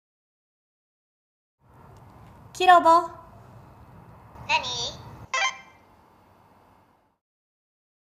話者方向推定
トヨタ自動車(株)（以下、トヨタ）は、コンパクトなサイズで常に寄り添うコミュニケーションパートナー「KIROBO mini」を全国のトヨタ車両販売店を通じて2017年に発売する予定。